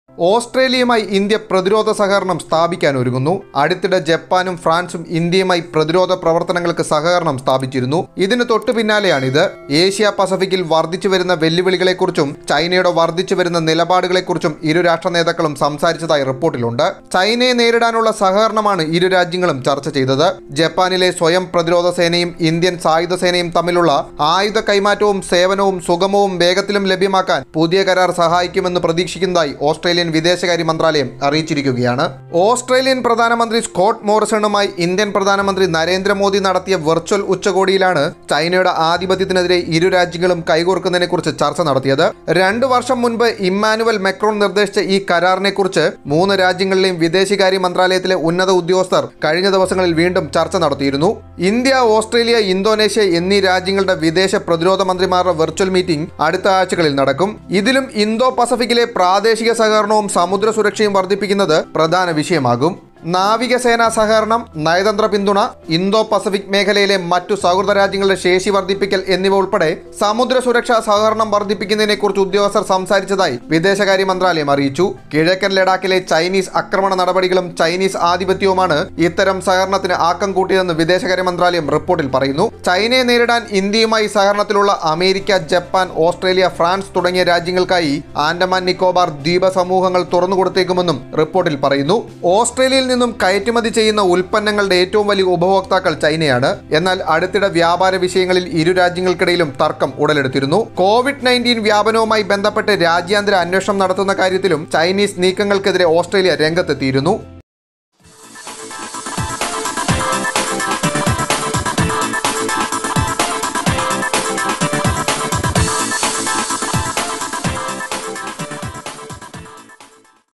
ഓസ്ട്രേലിയൻ സൈനികർക്കായി ഇന്ത്യ ആന്റമാൻ ദ്വീപ് തുറന്നു കൊടുക്കുന്നു..!! | ഇന്ത്യാ-ഓസ്ട്രേലിയ കരാർ...
ഓസ്ട്രേലിയൻ സൈനികർക്കായി ഇന്ത്യ ആന്റമാൻ ദ്വീപ് തുറന്നു കൊടുക്കുന്നു..!!
ചൈനയെ വെട്ടിലാക്കി ഇന്ത്യാ-ഓസ്ട്രേലിയ കരാർ...